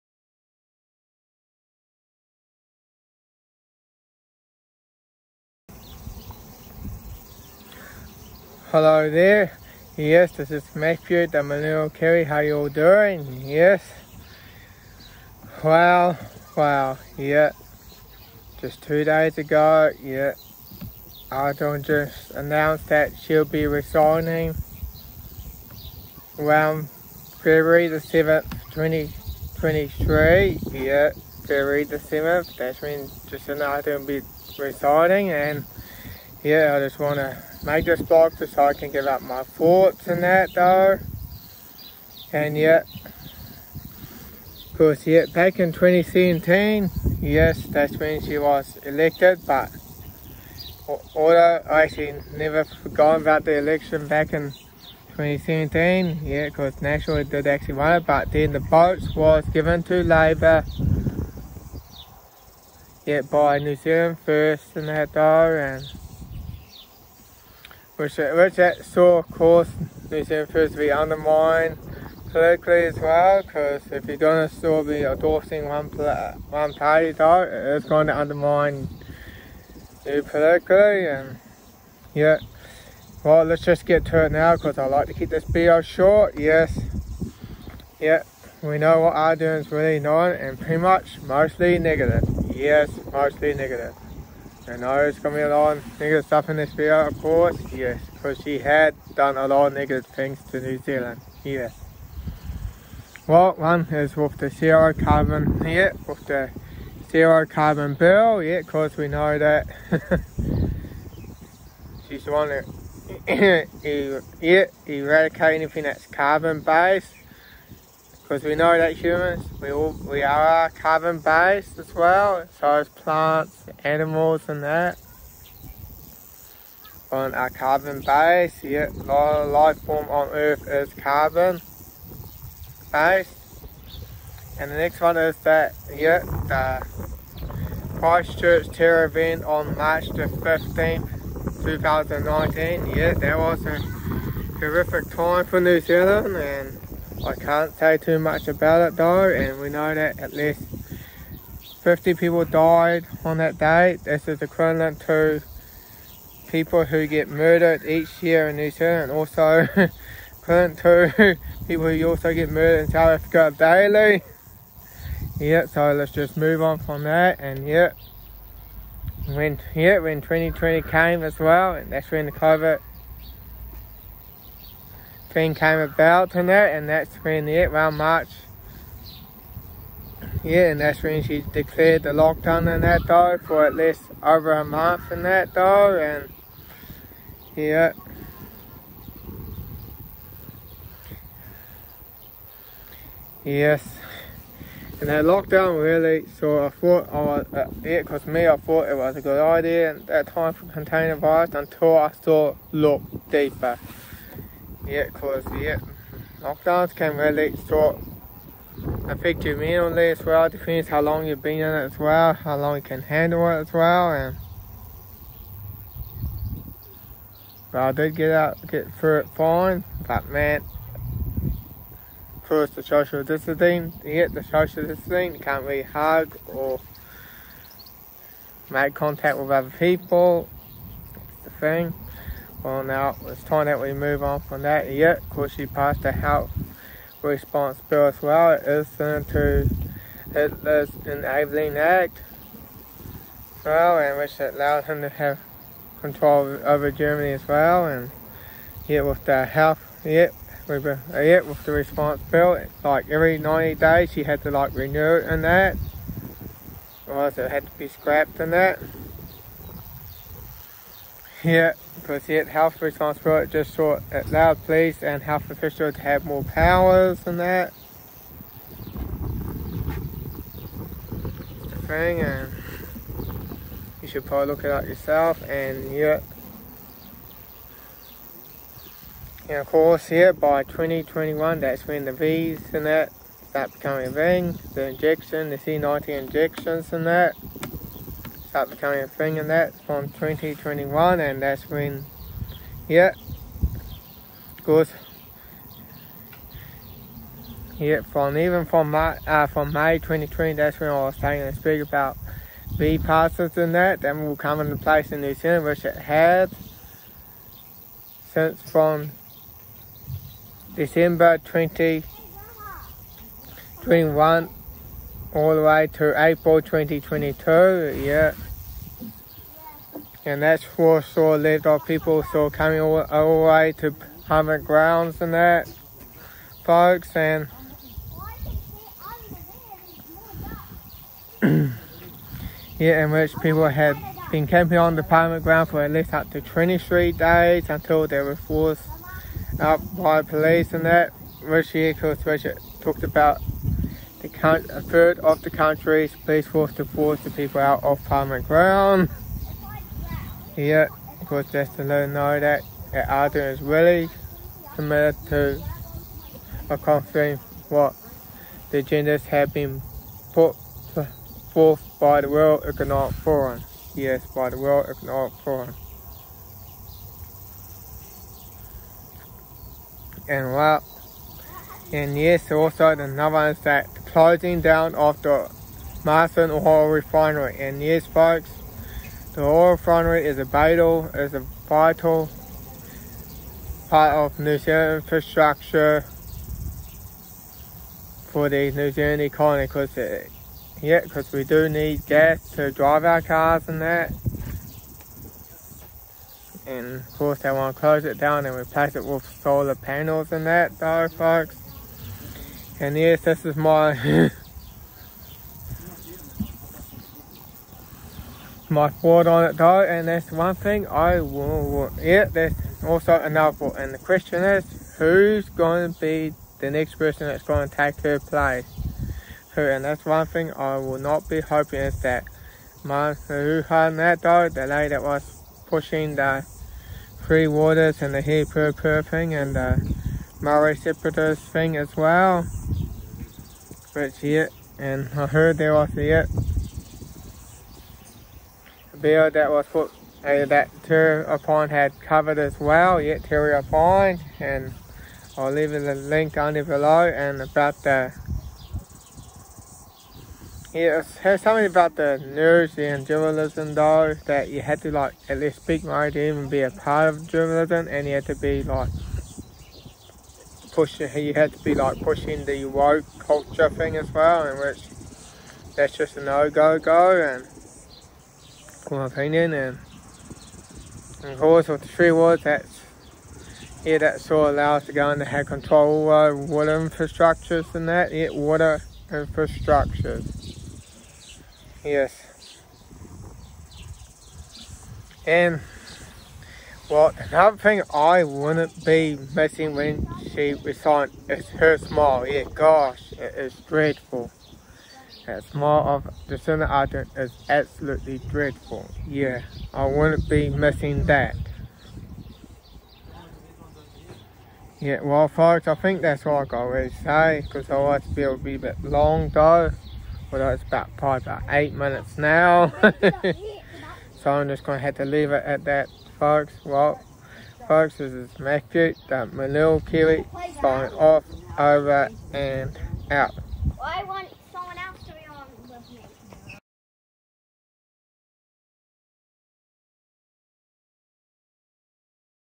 MK Vlog 21012023 My Thought on Jacinda Ardern Resigning
My thought on Jacinda Ardern Resigning and who going take her place Hipkins or Nahuta? These were my most likely people who take Ardern's place as Prime Minister of New Zealand.